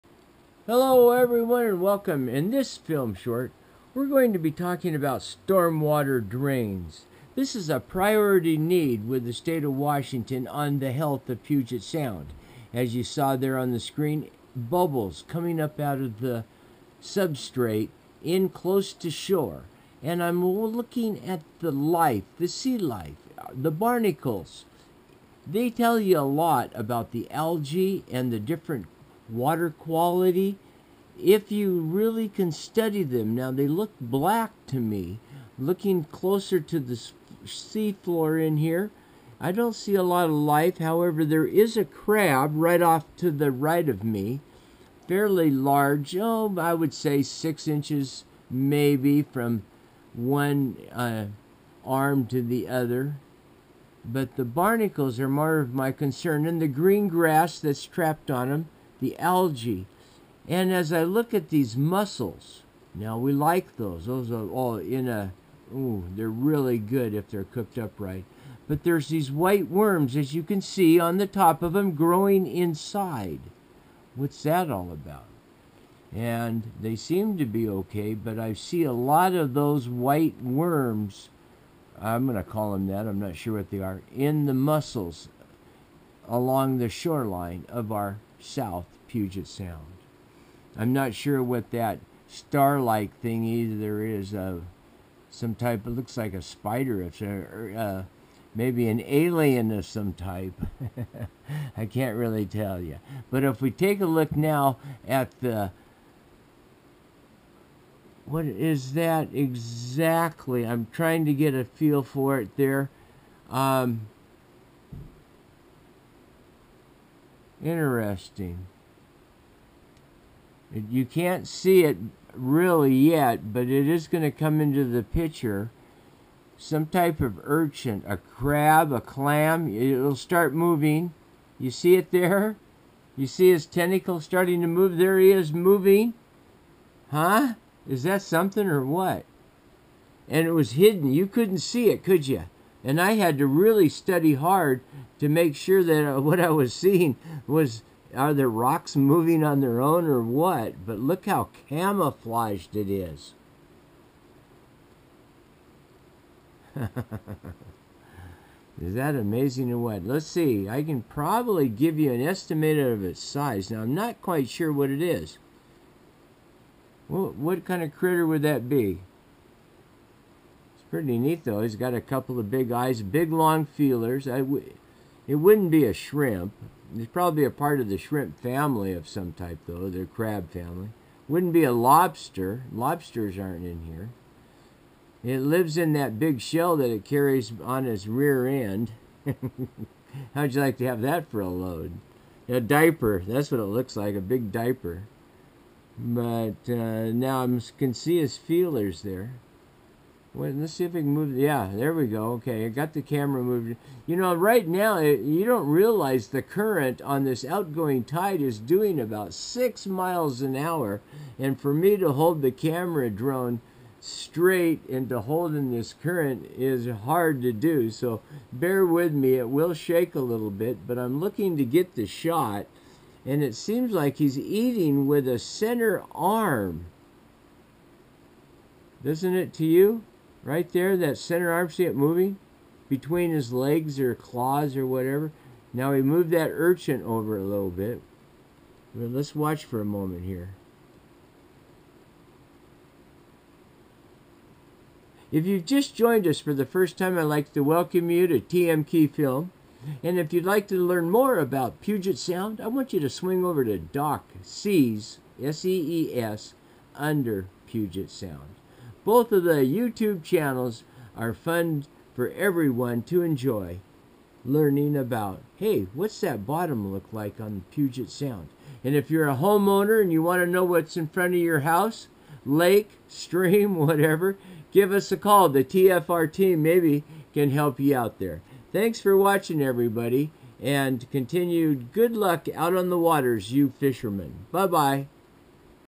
Storm water Drains Priority needed
This underwater Diver Drone video short following WA. contract of the 2222 environmental services. TFR  mission too A/B roll old video over 20 years to the new research recordings today. Instantly viewing the changes "Good and Bad." From the top of the trees to the bottom of the sea. Puget Sounds near shore marine sea life environment can be helped by public education.
They say, a photo says a thousand words. OK see for your self  changes taken place over the past 20 years.
Coming soon all FHD 4 K even higher and 360 viewing as we fly along the Deepest depths of the Puget sound recording the baby ground fish schools swimming over the sea floor in a depth of 590 ft. However; in this short we are recording the marine life in 8ft. of water on a out going tide moving at 6mph.
Your friend, Doc Thoke. AKA "Ghost Net Guy"